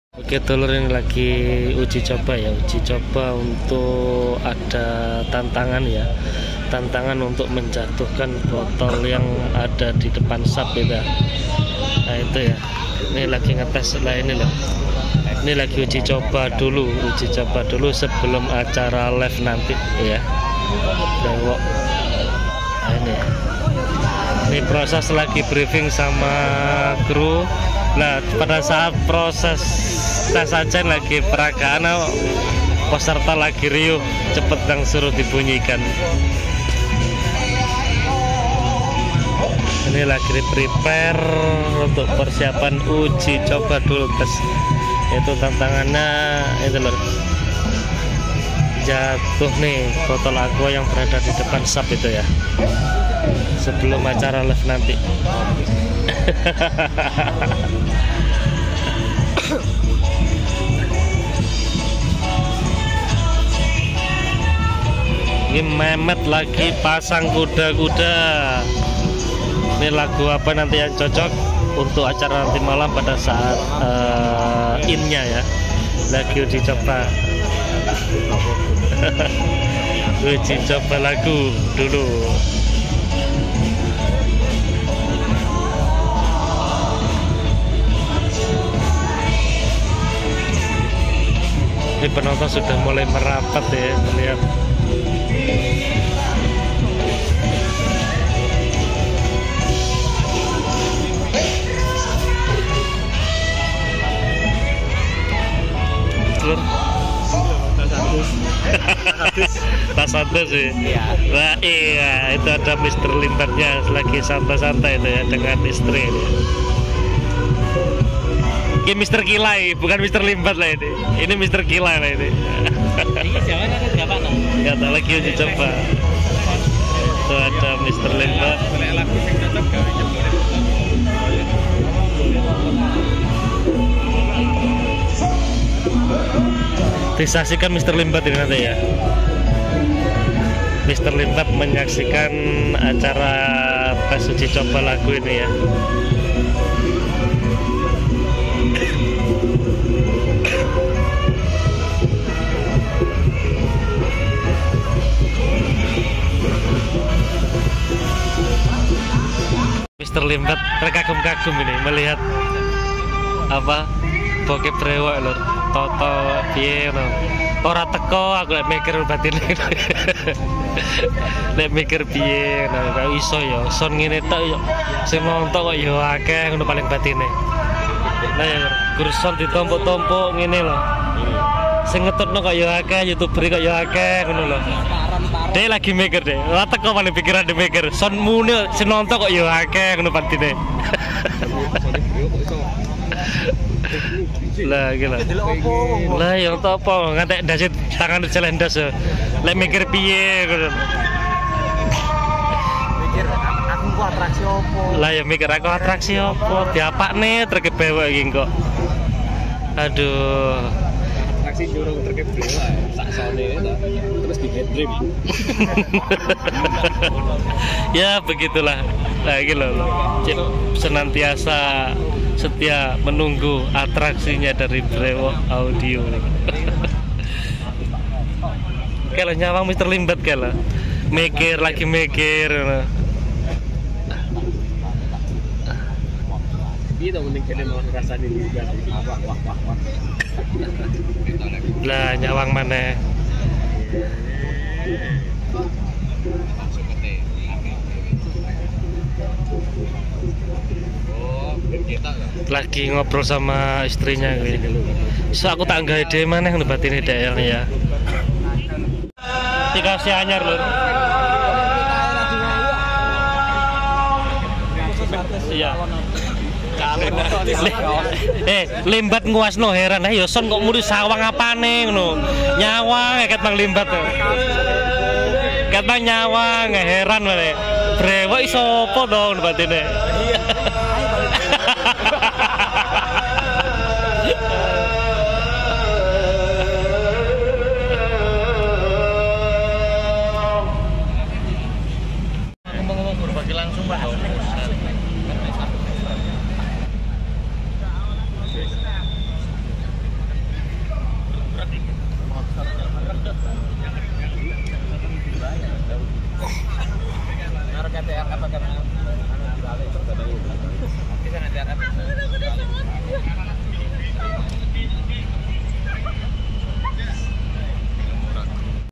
Master Limbad Terheran Heran Melihat Persiapan Brewog Dapat Tantangan Jatuhkan Air Dalam Botol
kilau mnc tv